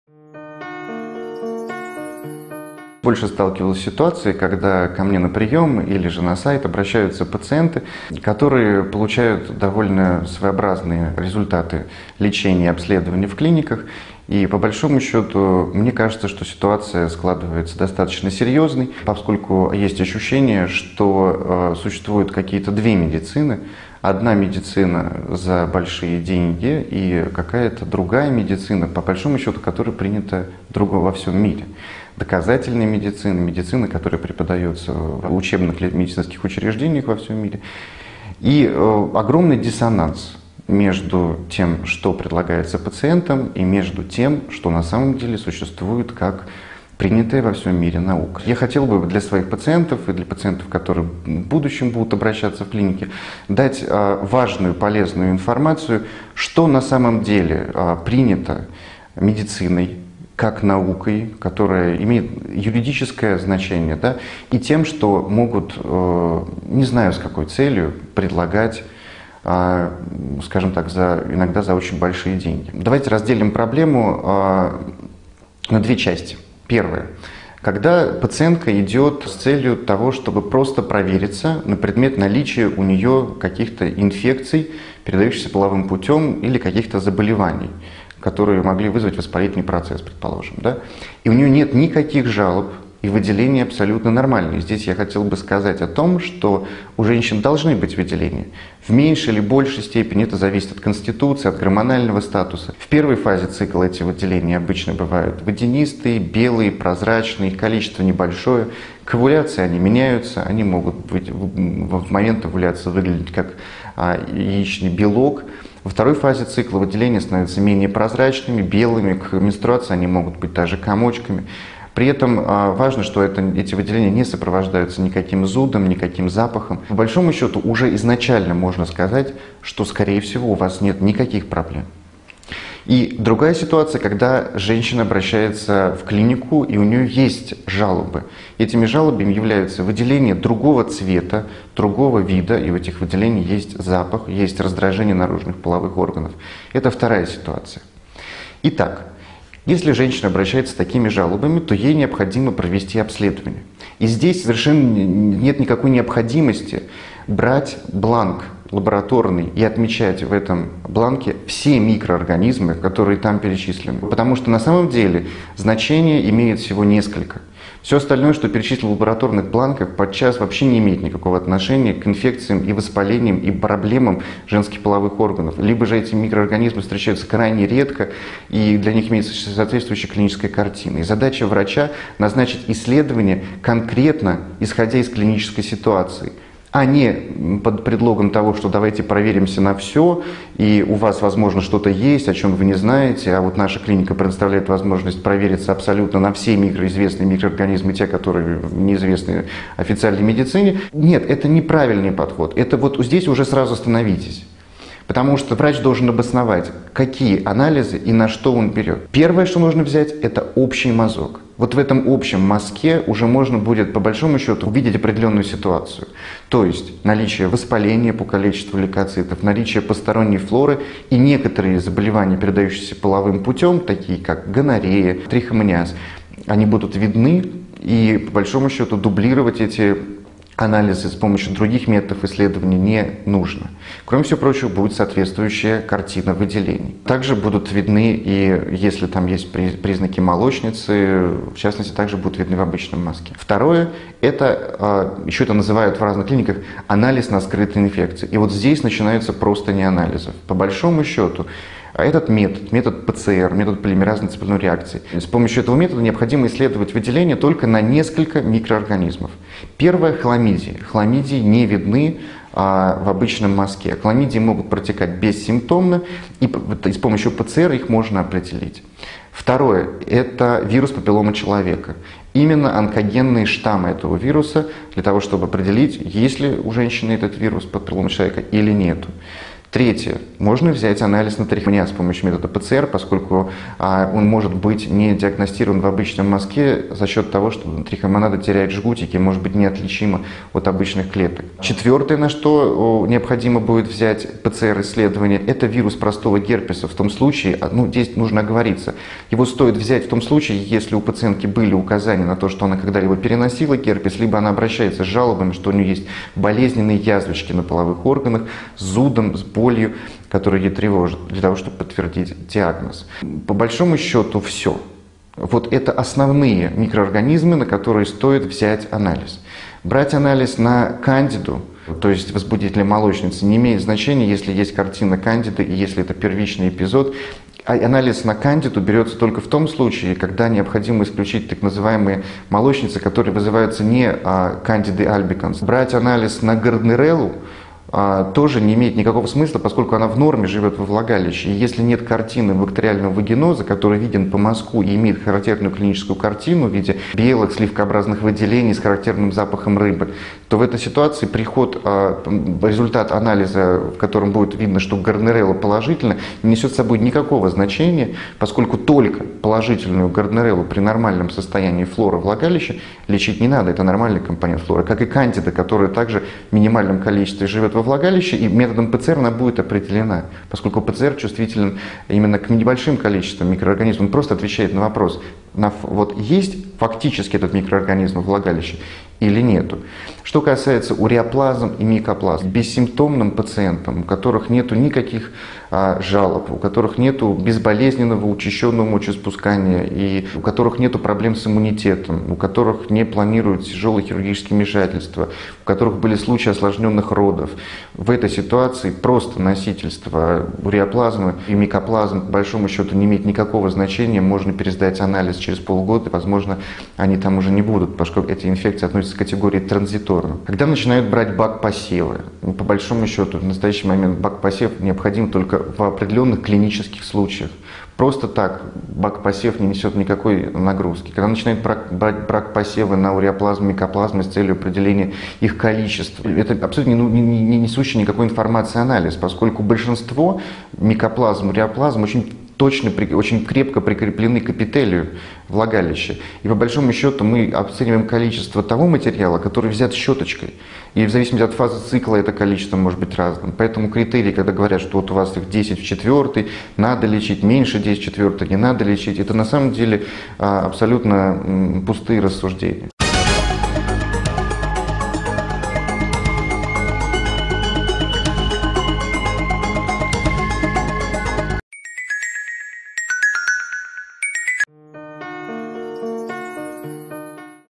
Нужные и лишние анализы в гинекологии
Довольно часто пациенткам назначают огромное количество анализов, большая часть которых не является информативной для диагностики подозреваемого заболевания. В первую очередь врач должен полагаться на клинические симптомы заболевания. Объем назначаемых исследований женщине должен четко соответствовать клинической картине.
Проверяться на все и вся - не стоит.
Наиболее важные в гинекологии анализы: общий мазок, анализ на скрытые инфекции, который проводится методом ПЦР. К анализам на скрытые инфекции относят
- выявление хламидий - так как их не видно под микроскопом в общем мазке,
- вирус папилломы человека - только онкогенные штамы 16,18,
- трихомонады,
- вирус простого герпеса - только при наличии соответствующих жалоб и ранее перенесенного генитального герпеса.